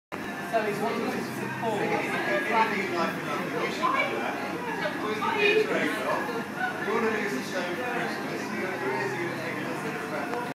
Striding out on alter G treadmill 6.10.11
Just look at those strides! Yet more progress, wearing rigid AFO.